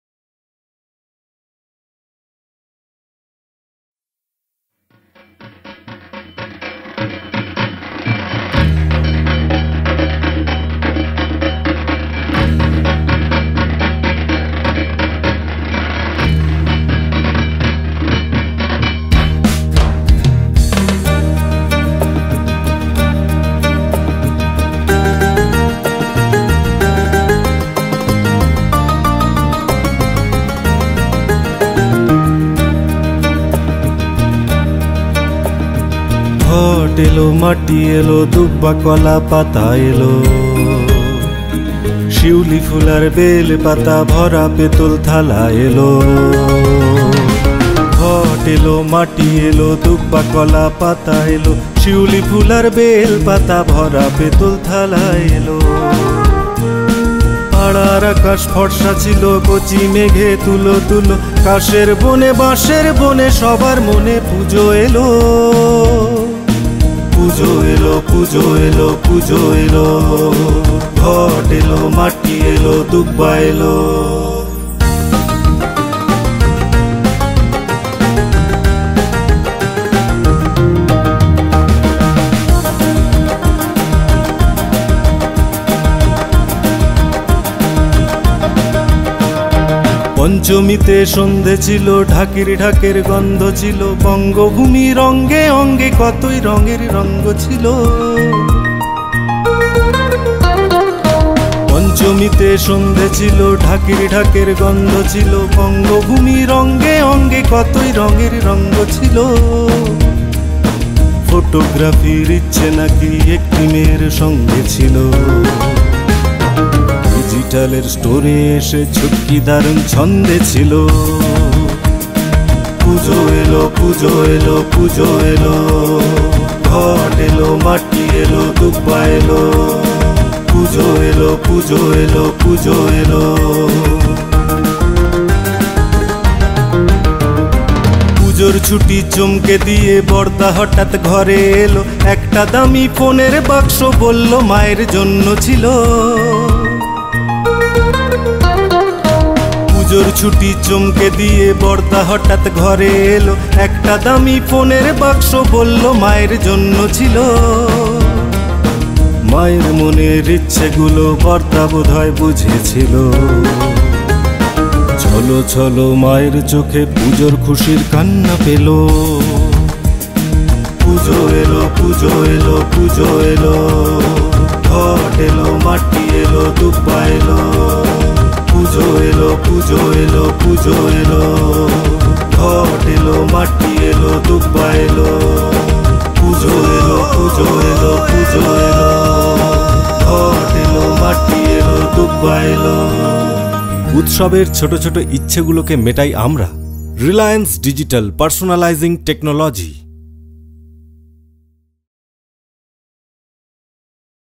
Celebrate Pujo with Reliance Digital
We're reveling in the spirit of Pujo! Whether it is buying a special gift for your loved ones or starting a new journey together, Reliance Digital has been a part of every celebration! Come celebrate Pujo with us at the FestivalOfElectronics!